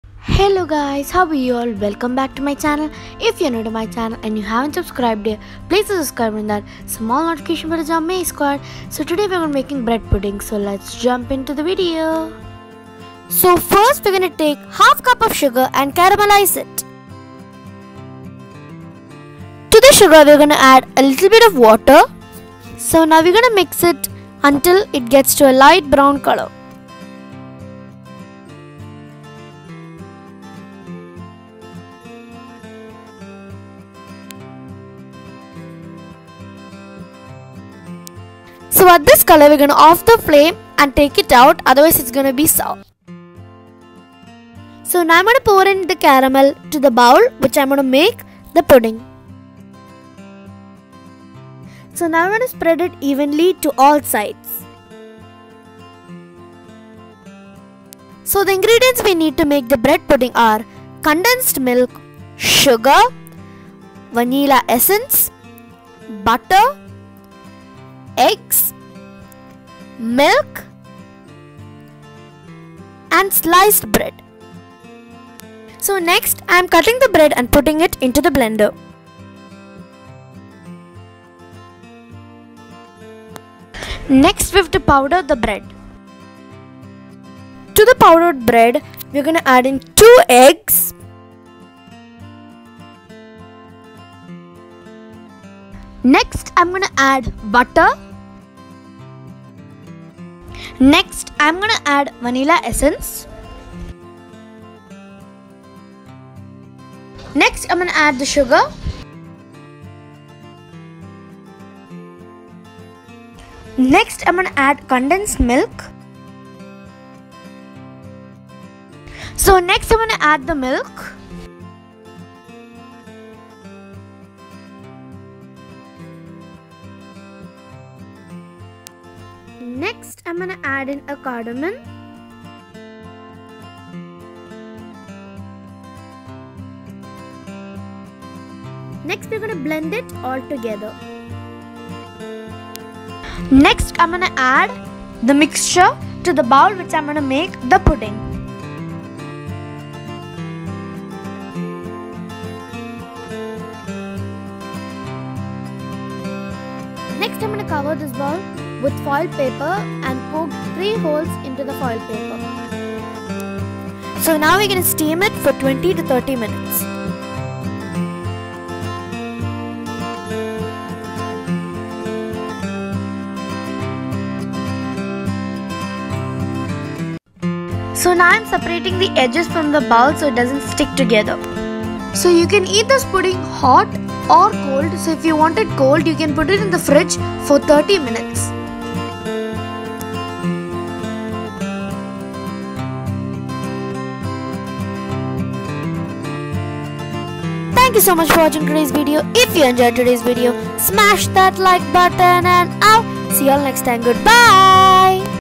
🥰🥰Quick & Easy Bread 🍞 Pudding🥰🥰
Episode 7😃

Recipe- Amma’s

Hi guys.

Ingredients-
Bread- 6 slices
Vanila essence- 1 1/2 teaspoon
Butter-50 grams
Sugar-3 tablespoon
2 eggs